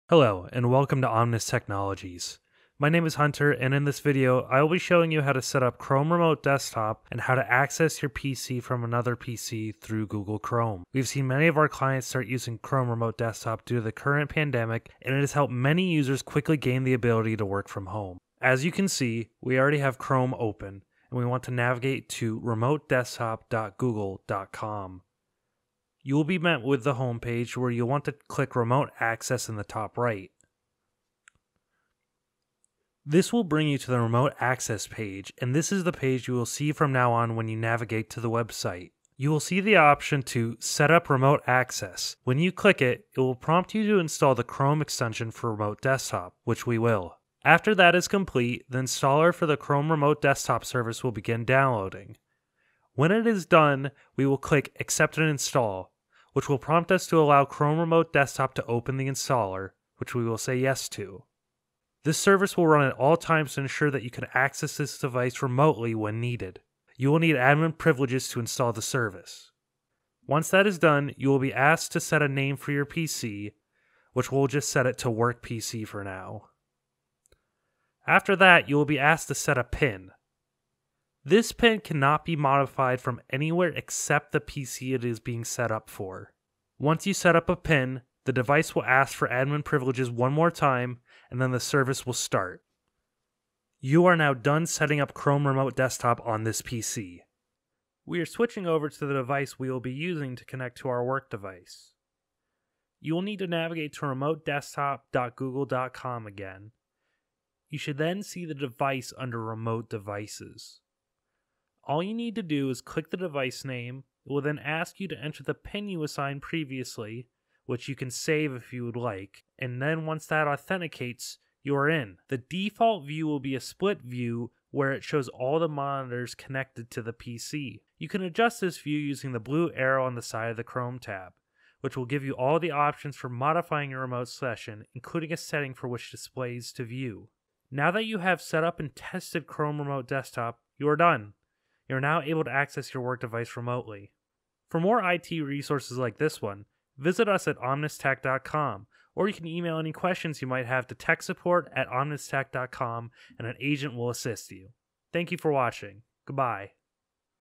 How to Access Your Work PC From Home - Chrome Remote Desktop Tutorial
Chrome Remote Desktop is a free service provided by Google that lets you access your work PC from home. It’s a solution we recommend for quickly and securely accessing your work device from home or while you’re traveling.

Omnis Technologies provides professional IT support in Bradford, PA and surrounding areas.

Connect with Omnis Technologies